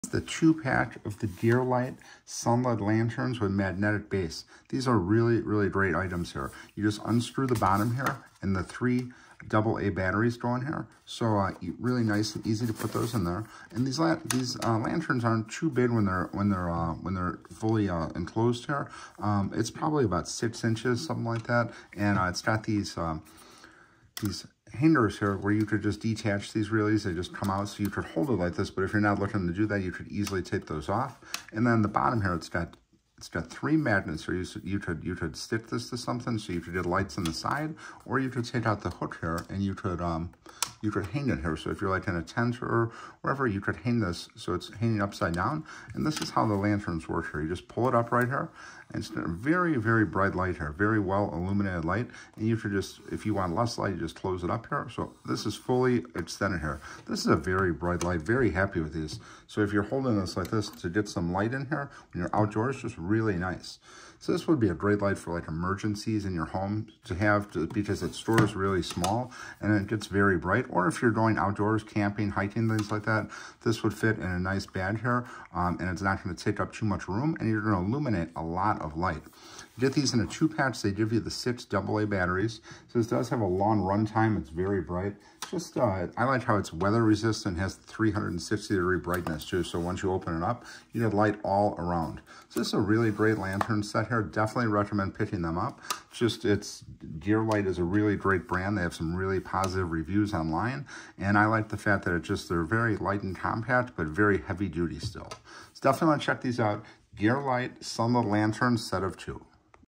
GearLight Portable LED Camping Lanterns - Battery Powered
Review of the GearLight Camping Lantern - 2 Portable, LED Battery Powered Lamp Lights, Magnetic Base and Foldable Hook for Emergency Use or Campsites.